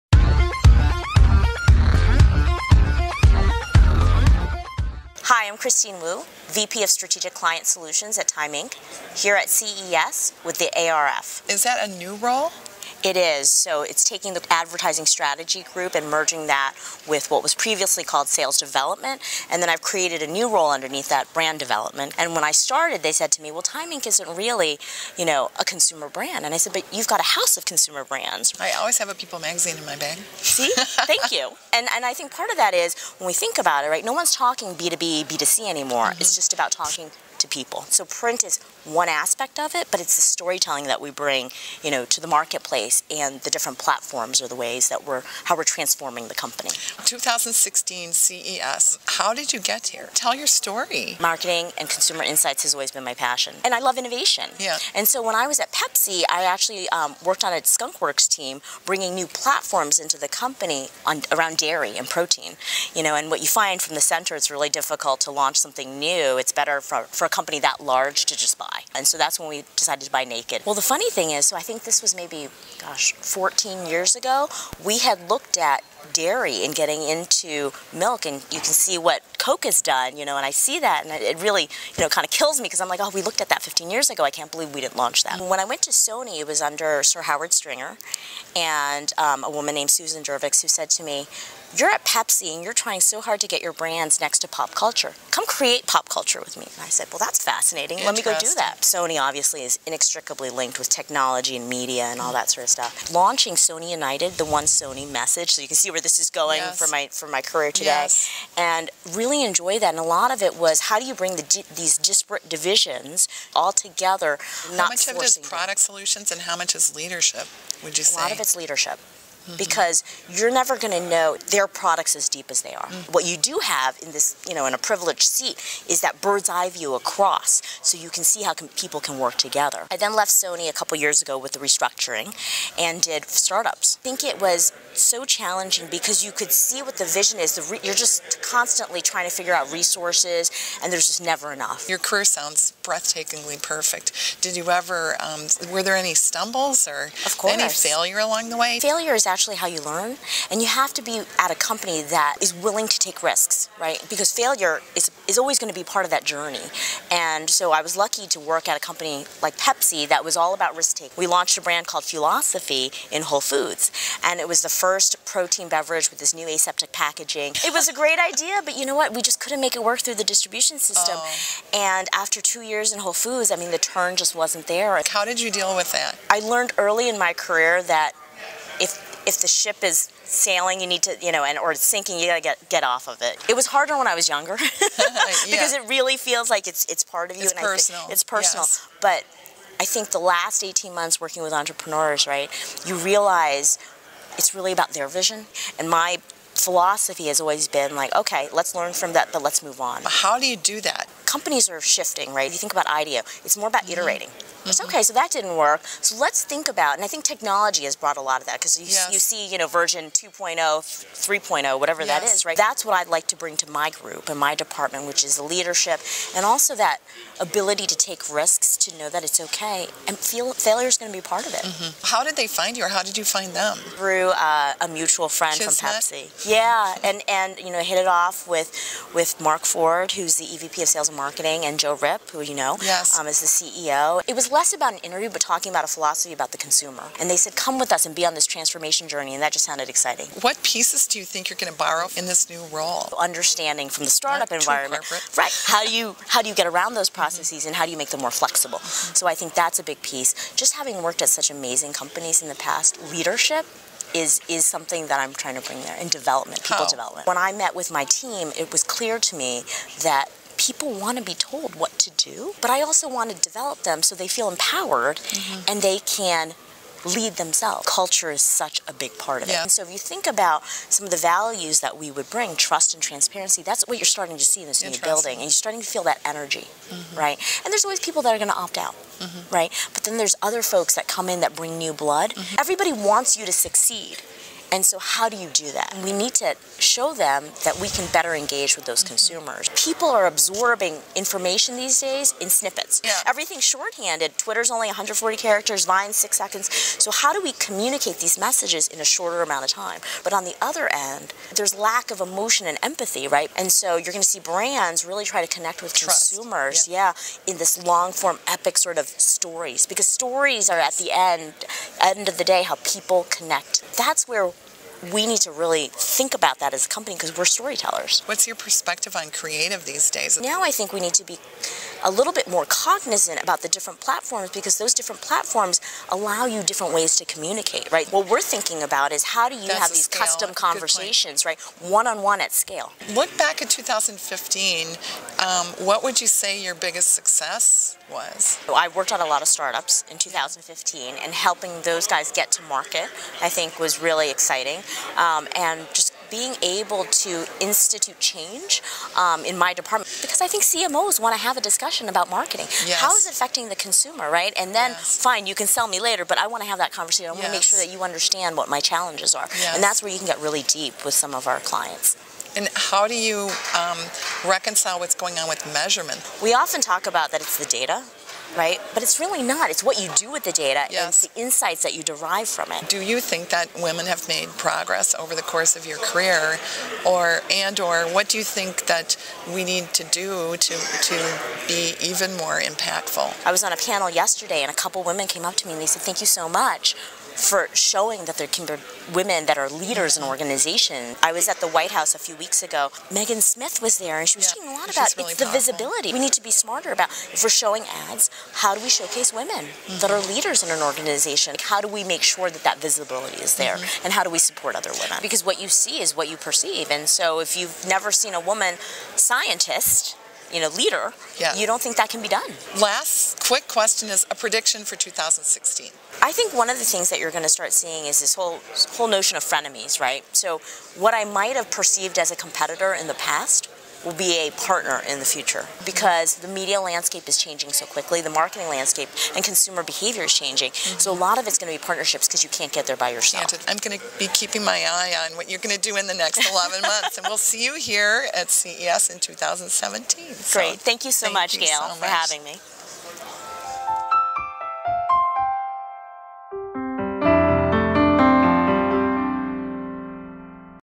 ARF 5 Mins Interview @ CES with Christine Wu from Time Inc
Vice President, Strategic Client Solutions
Time Inc.